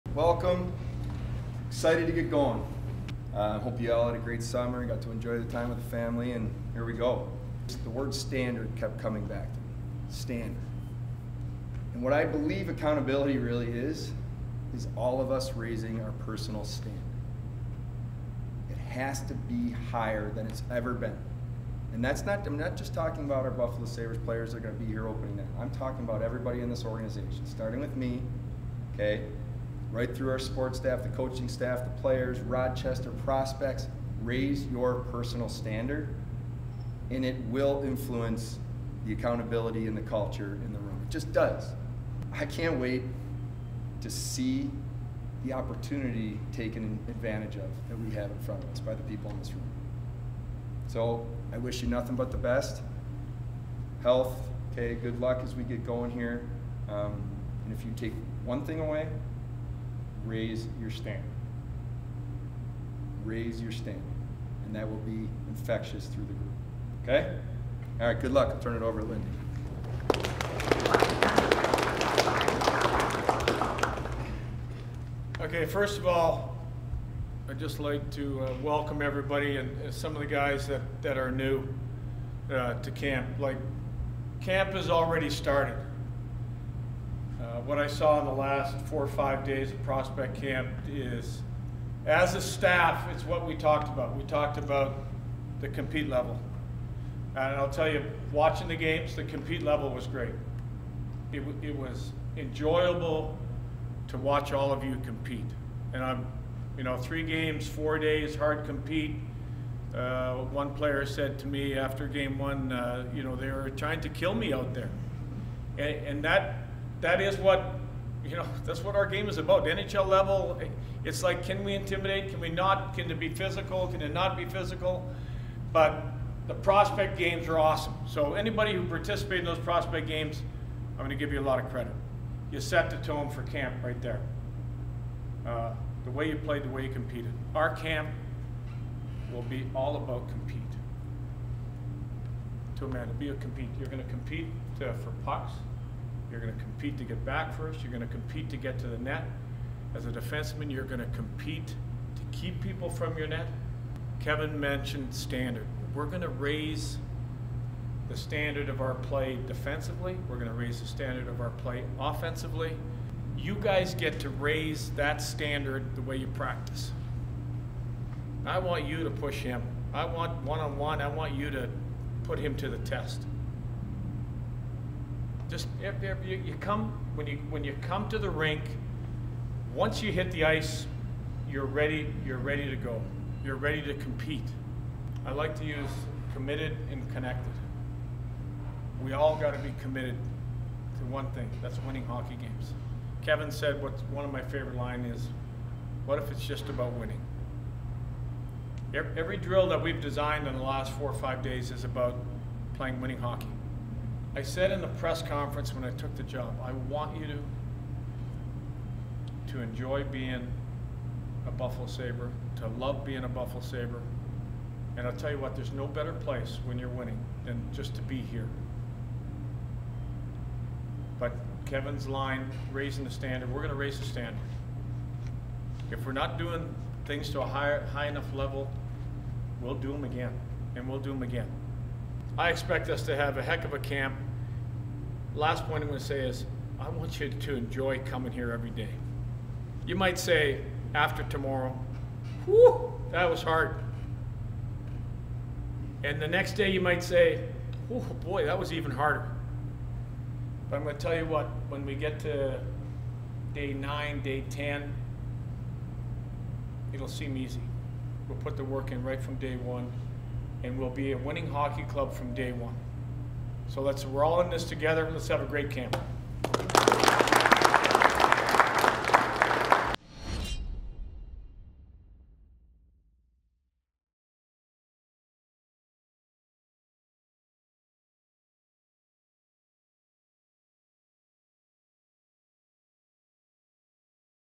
"There's Nothing Better" | Kevyn Adams and Lindy Ruff Address Buffalo Sabres At Training Camp
Buffalo Sabres general manager Kevyn Adams and head coach Lindy Ruff addressed the attendees of Sabres 2024 Training Camp. They discussed what the expectations would be during camp, the pace, and what it means to be a Buffalo Sabre.